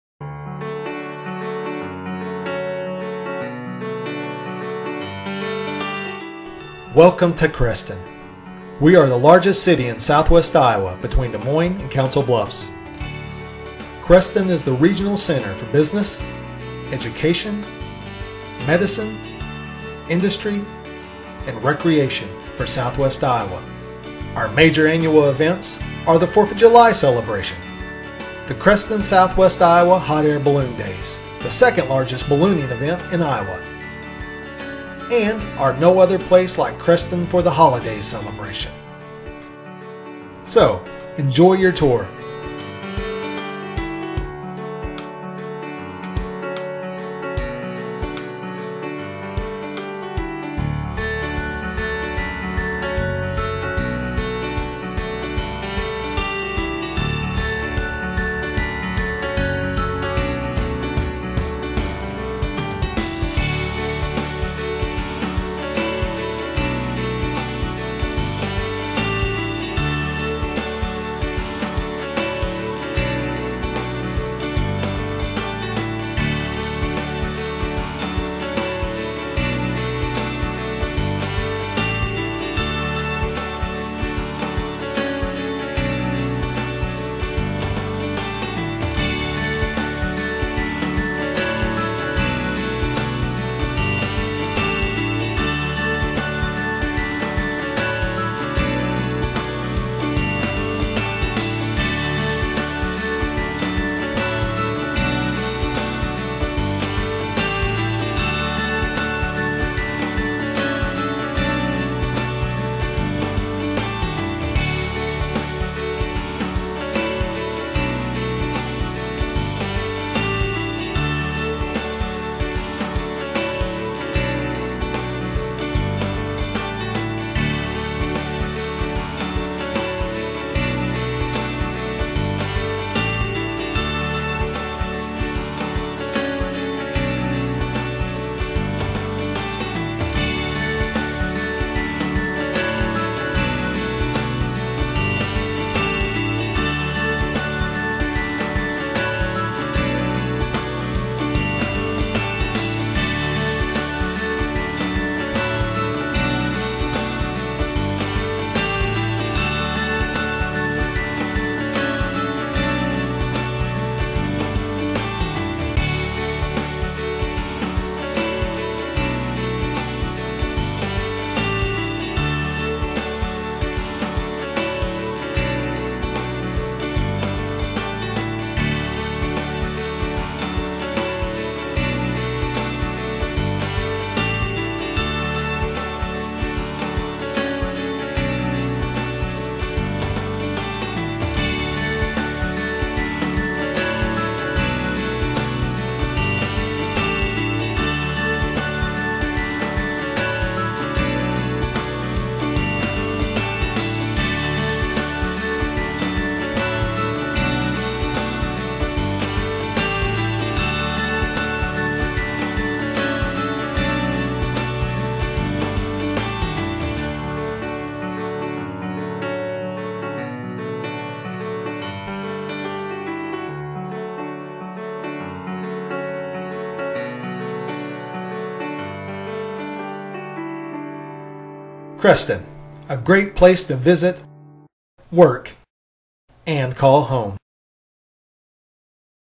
Creston, Iowa video tour
The Creston Chamber of Commerce presents the Creston, Iowa video tour